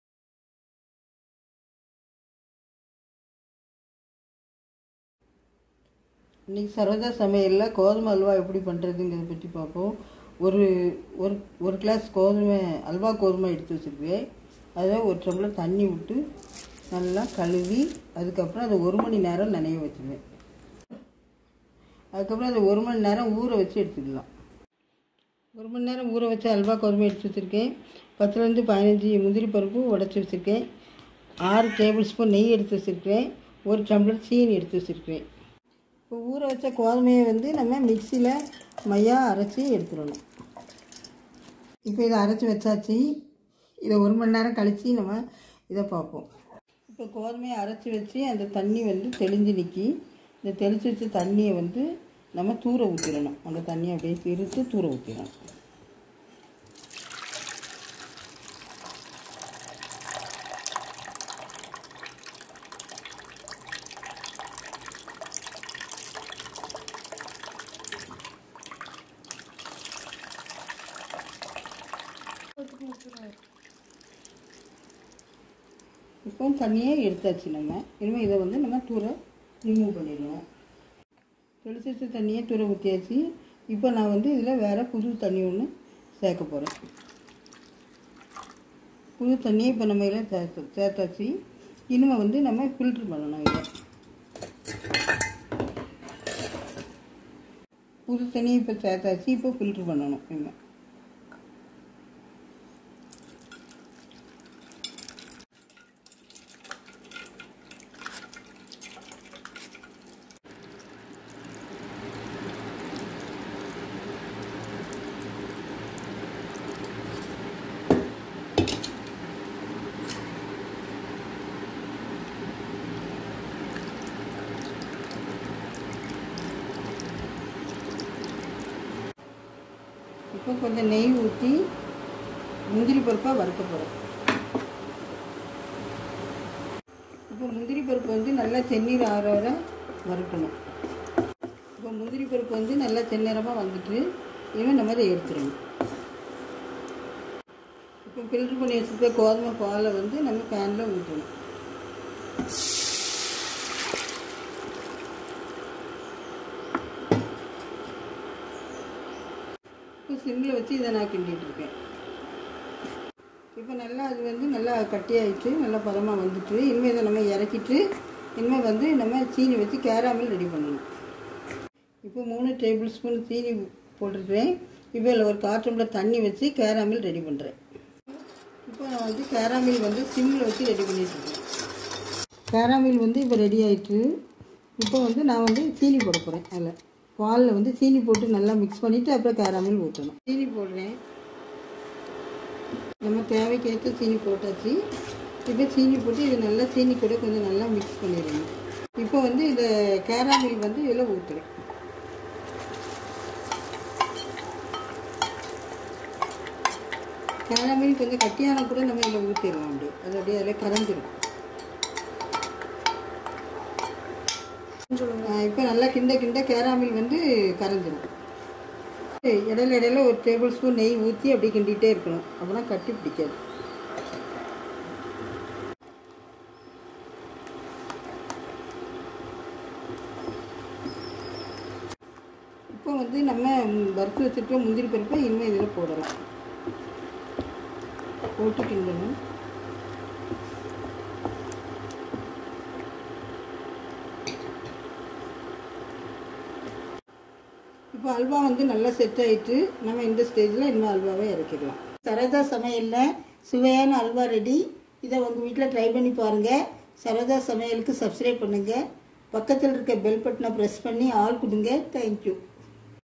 Wheat Halwa,/ Halwa,/ Halwa in Tamil
Ingredients

Halwa Wheat-1glass
Cashew Nuts- 10 to 15
Sugar - 1glass
Required Ghee